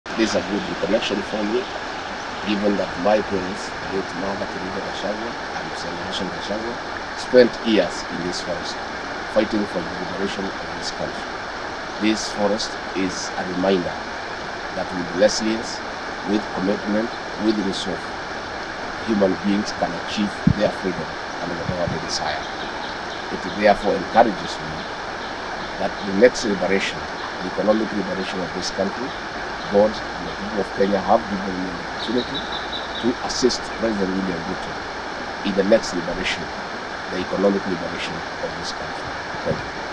Mt Kenya Forest is a reminder human beings can achieve freedom, DP Gachagua says
Mt Kenya Forest is a reminder that with commitment and resolve human beings can achieve freedom, Deputy President Rigathi Gachagua says.